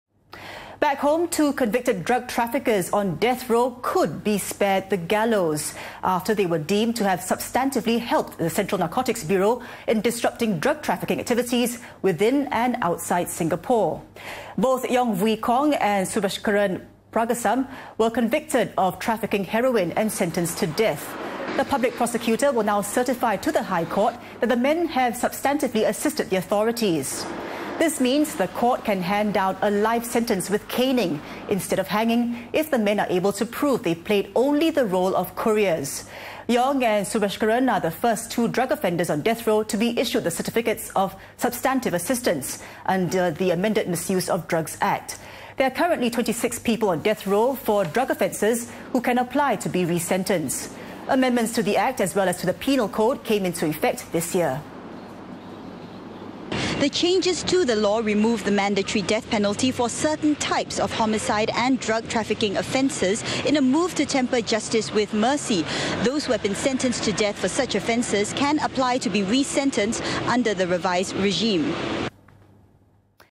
Two convicted drug traffickers may be spared death penalty - 18Sep2013
SINGAPORE: Two men currently on death row for drug trafficking may be spared the death penalty after they were deemed to have substantively helped the Central Narcotics Bureau in disrupting drug trafficking activities within and outside Singapore.

Yong Vui Kong was convicted by the High Court in 2008 of trafficking 42.27 grammes of heroin and given the death sentence, which is mandatory for offences involving more than 15 grammes of the drug.

Subashkaran Pragasam was sentenced to death in October last year for trafficking 186.62 grammes of heroin.

Yong had previously made several petitions against his sentence.

In a statement on Wednesday, the Attorney-General's Chambers (AGC) said the Public Prosecutor will now certify to the High Court that both men have substantively assisted the authorities.

This means that instead of the death penalty, the court can hand down a life sentence with caning - if they are able to prove they only played the role of couriers.

Yong and Subashkaran are the first two drug offenders on death row to be issued the certificates of substantive assistance - under the amended Misuse of Drugs Act.

In a statement, M Ravi, the lawyer who is acting for Yong, said that he will be applying to the courts for his client to be re-sentenced.

"This news comes as a tremendous relief to me," he said.

Tan Chuan Thye, who is acting for Subashkaran, noted that this is very much the first step, and a case management hearing has been set for next month. He added that he intends to do his best for Subashkaran.

The AGC said there are currently 26 people who have been sentenced to death for drug offences who can apply to be re-sentenced under the new regime

It said it will review every case where the person wishes to apply to be re-sentenced under the new regime.

Amendments to the Act, as well as to the Penal Code, came into effect this year.

The changes to the law removed the death penalty for certain types of homicide and drug trafficking offences, in a move to temper justice with mercy.

Those who have been sentenced to death for such offences can apply to be re-sentenced under the revised regime.